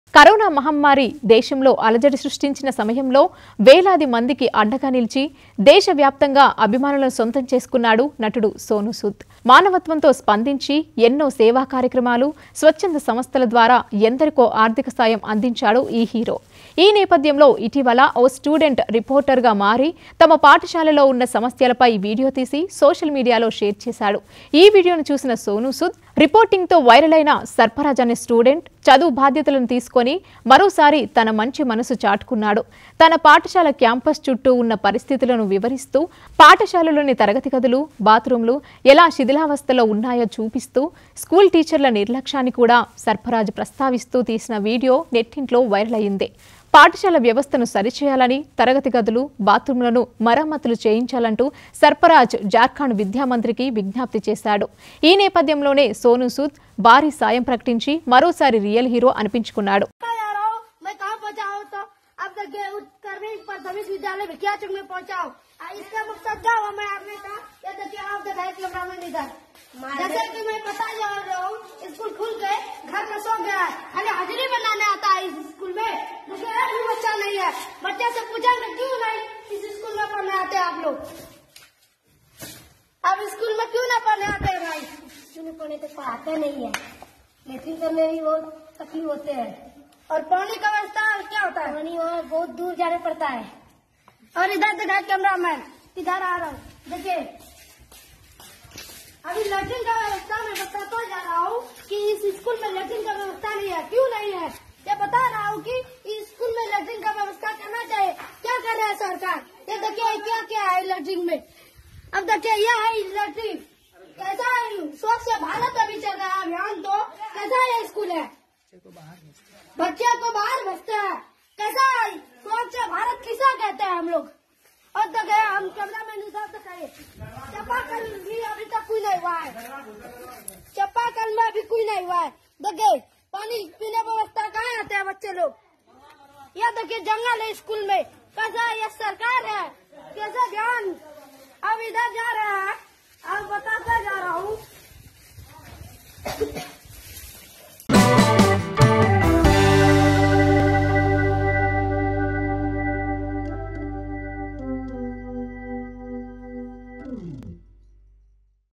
Sonu Sood Reacted on Jharkhand Student Ground Reporting on his School Bad Condition | TV5 Tollywood
Sonu Sood reacted on jharkhand Student ground reporting on his school bad condition viral video | TV5 Tollywood

actor sonu sood offer education to 12 years old Student sarfaraz ground reporting on his jharkhand government school bad condition viral video